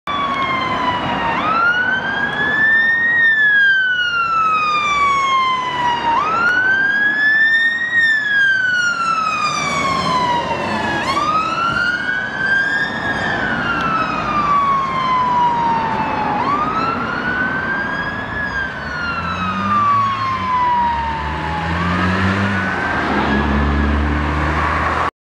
NIFRS / S2601 / ERZ 7966 / Volvo FLL / WrL / Responding
Newtownards based Water Rescue Ladder seen responding to an emergency in Bangor.